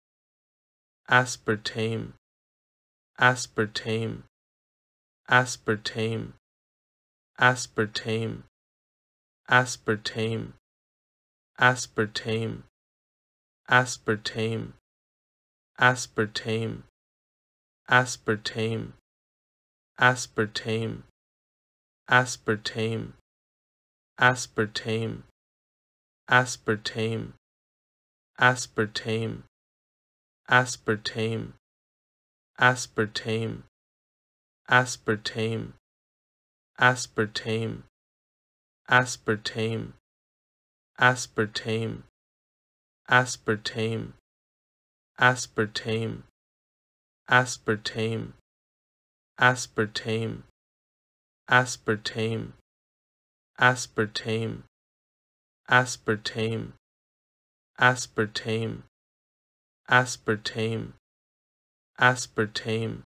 How to pronounce Aspartame||How to say Aspartame||Aspartame Pronunciation||ABDictionary
Do you want to know how to pronounce Aspartame? In this video you'll learn how to say or pronounce Aspartame for free